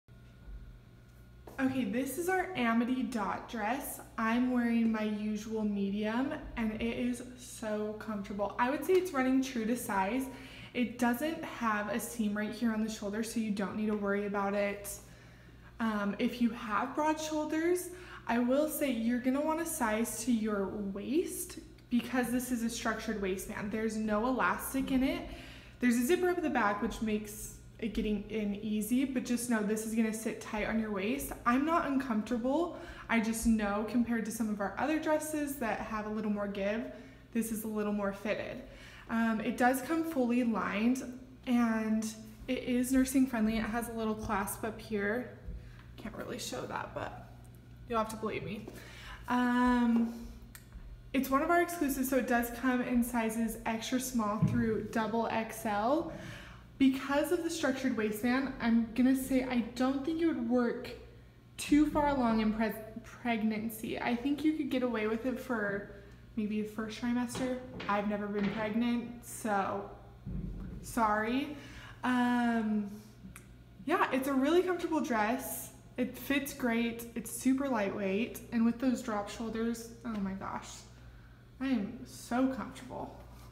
Piper & Scoot: The Amity Dot Wrap Midi Dress in Black
The Amity Dot Wrap Midi Dress is a black dress in a midi length with an intricate tiny dot pattern. It is our latest exclusive, so it does come in extended sizing. This dress features a wrap style top, complete with a clasp, which makes it nursing friendly. The structured waistline provides definition while the zipper in the back allows easy access getting into the dress. The dolman style short sleeves gives you complete freedom to move your arms without feeling any restrictions. This dress will be one you can wear today, tomorrow, and forever!